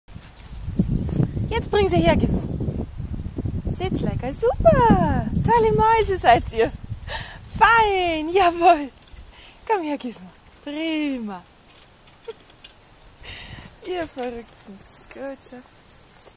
Bollerwagen
Gizmo zieht den Bollerwagen, Laika darf mitfahren...